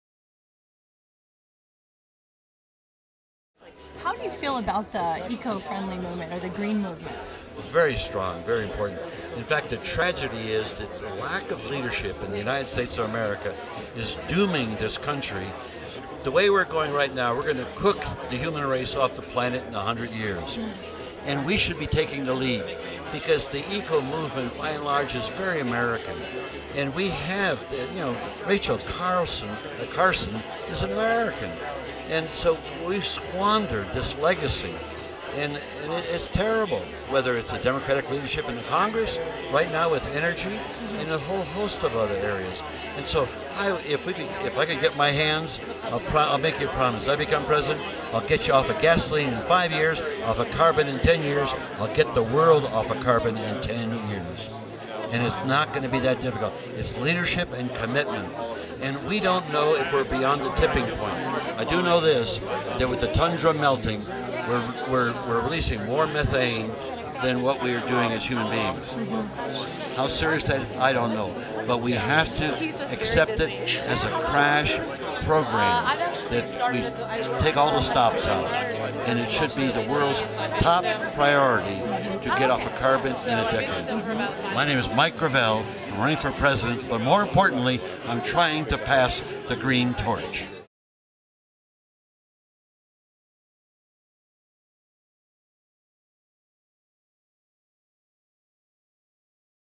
Mike Gravel - Passing the GreenTorch
Presidential candidate Mike Gravel passes the Green Torch at the 2008 Sundance film festival.